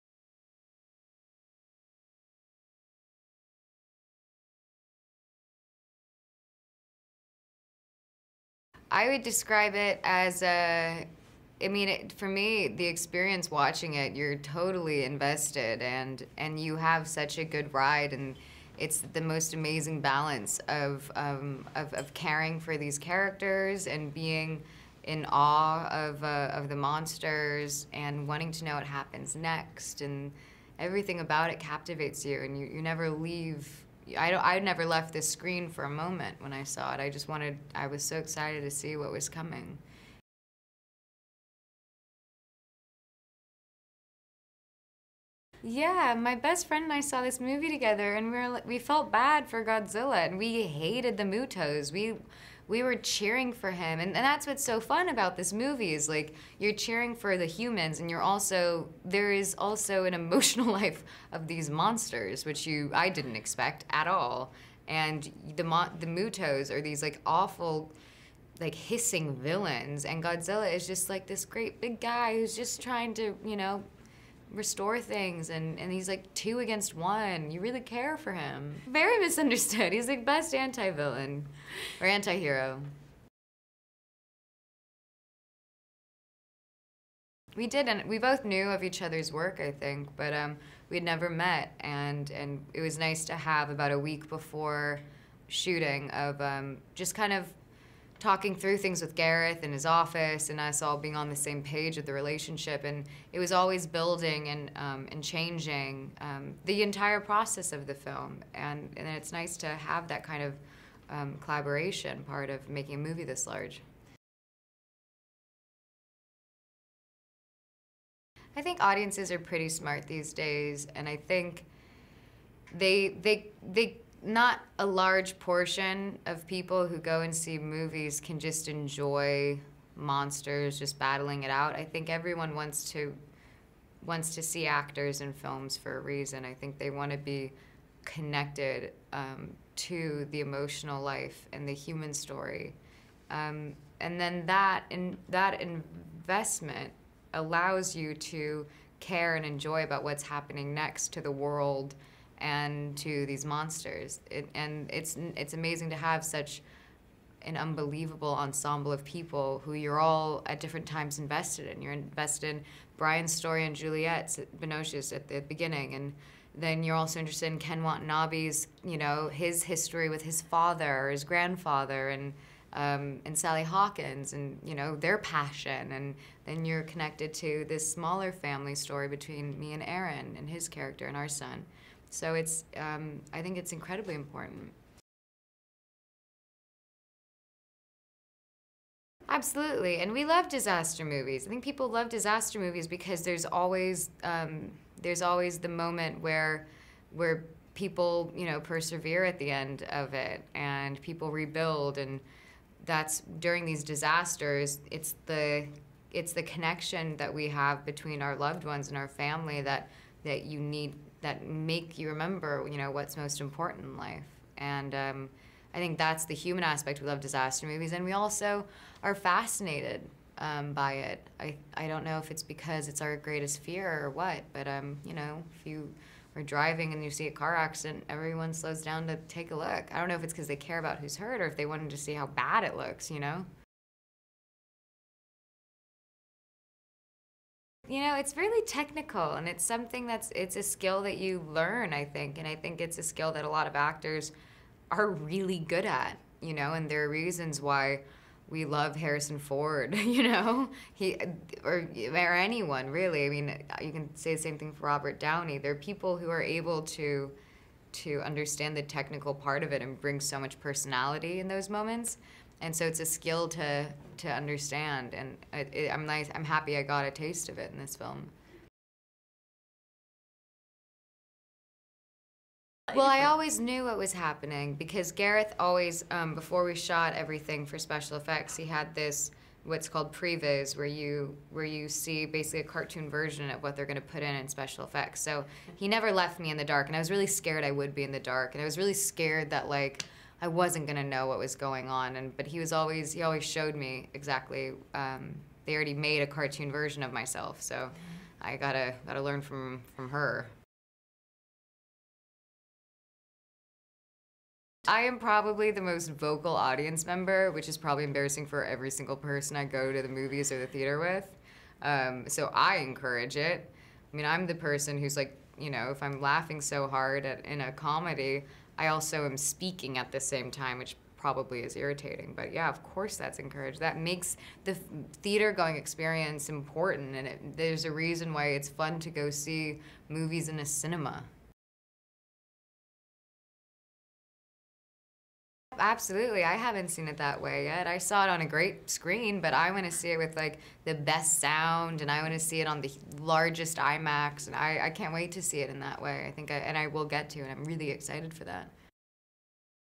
GODZILLA Entrevista ELIZABETH OLSEN
Entrevista de la actriz protagonista de "GODZILLA"a Elizabeth Olsen.

Godzilla ¡Ya en cines!